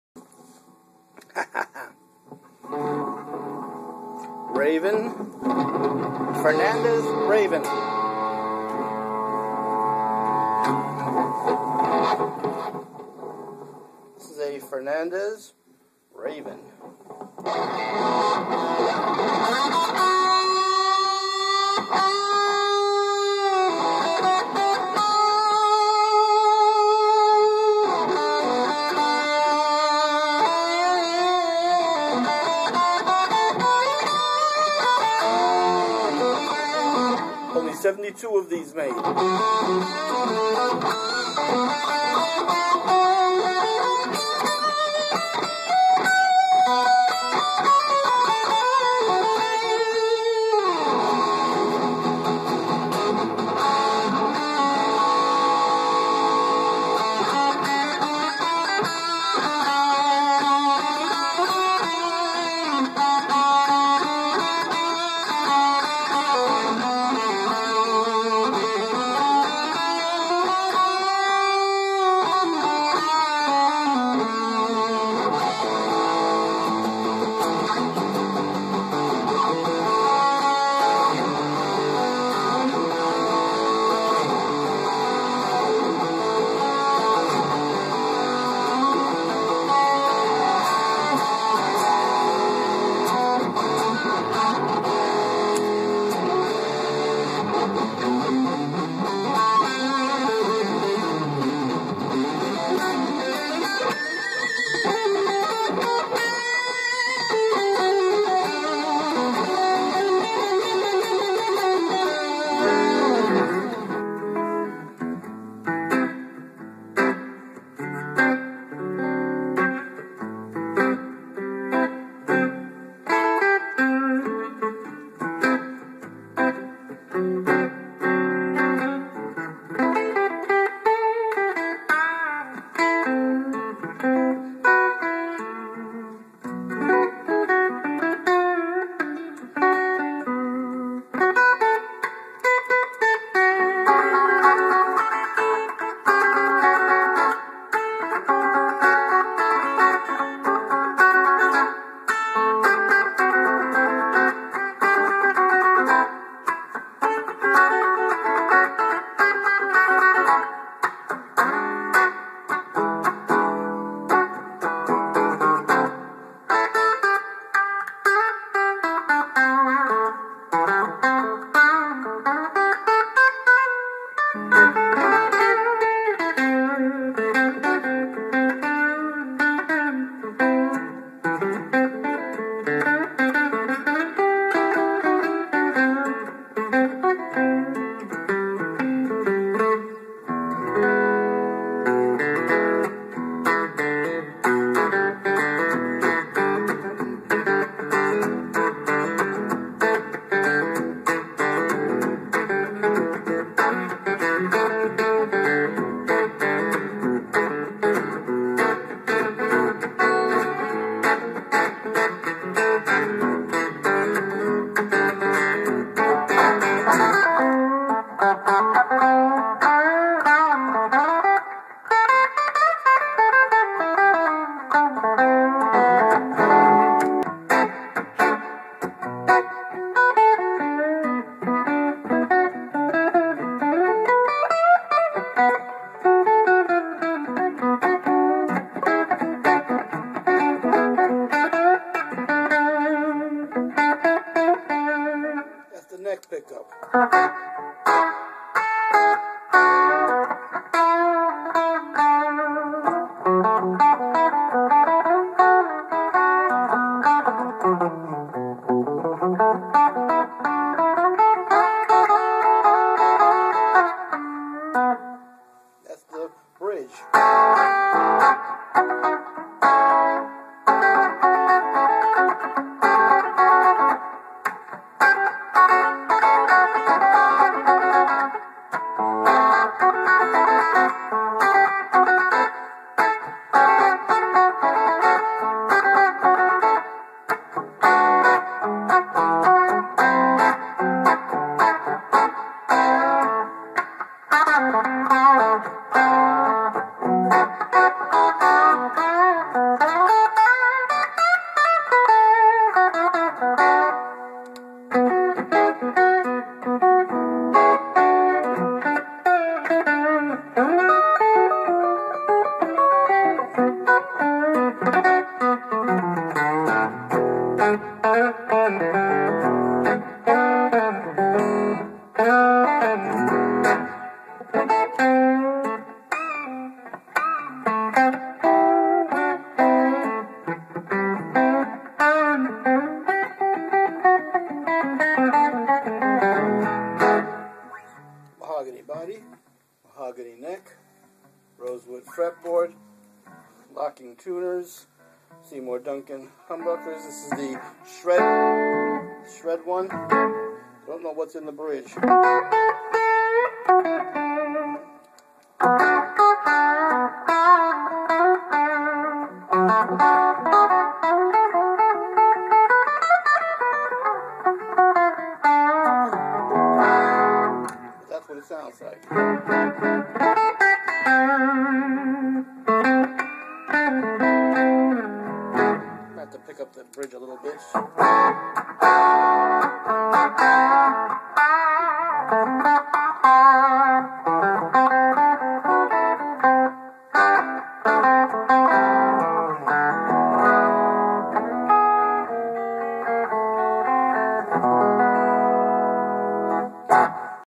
Fernandes Raven LTD demo
Only 72 of these were made back in 2004. The neck pickup is a Seymour Duncan Full Shred and the bridge pickup is a Seymour Duncan JB. Got it in a trade.